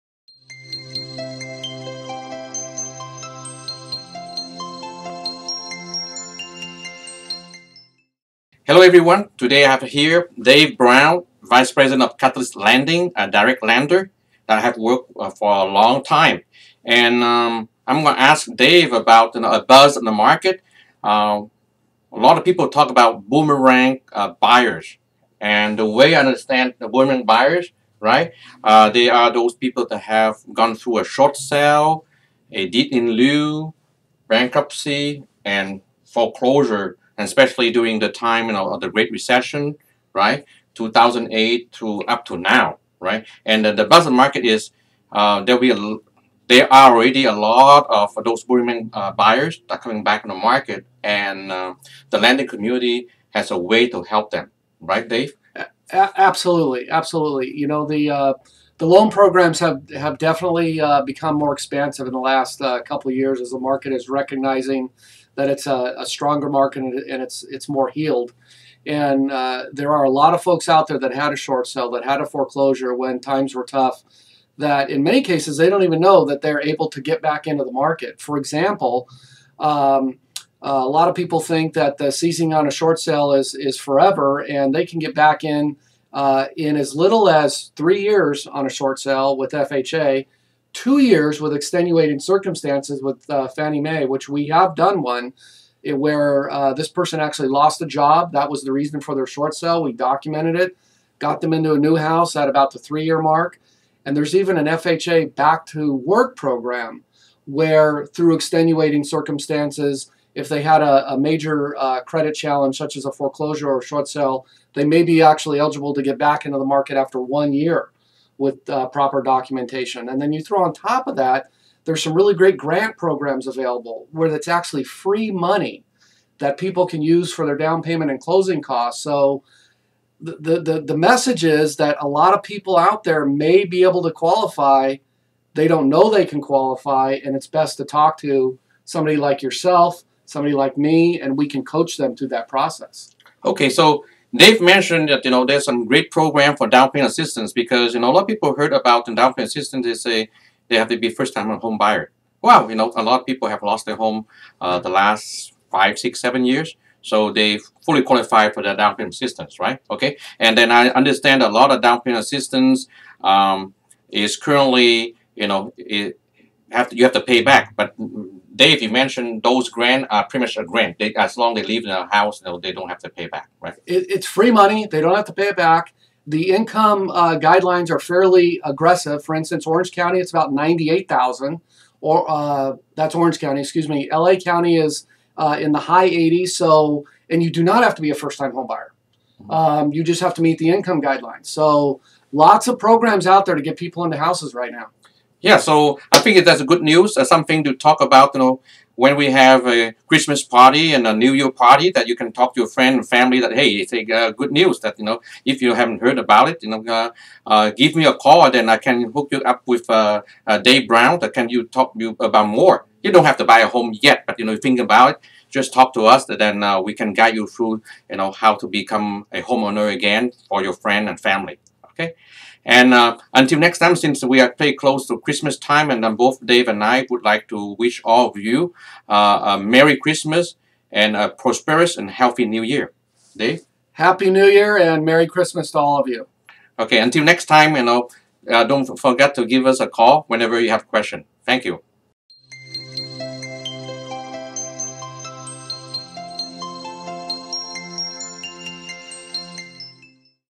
Are You Eligible to Come Back into the Southern California Market?
Today, Dave, our preferred lender, is here to talk about boomerang buyers. These are people who have gone through a short sale, bankruptcy, or foreclosure. Some of them may not even know they can come back on the market. To learn more, watch this short video!

Buying a home?

Selling a home?

Shawn Luong
Century 21
Southern California

Loan programs improved in the last couple of years. Plus, the market became stronger and healed. A lot of those buyers don't know they can come back into the market because of programs offered by the lending community.

Many clients think that the seizing on a short sale is forever. However, you can get back in the market in as little as three years through FHA or two years with Fannie Mae. There is also an FHA back to work program that allows people with major credit challenges to get into the market in one year.

Additionally, there are several great grant programs available to boomerang buyers. This is free money that you can use for a down payment or closing costs! A lot of people may be able to qualify, so talk to a professional to see which program works best for you.

You don't have to be a first-time home buyer to qualify for these down payment assistant programs. With grant programs, you don't even have to pay the money back.

Keep in mind income guidelines are extremely aggressive with grants. For instance, in Orange County, it's about $98,000. In LA County, it's $80,000.

Ultimately, these opportunities are great for people looking to enter the market. We look forward to hearing from you!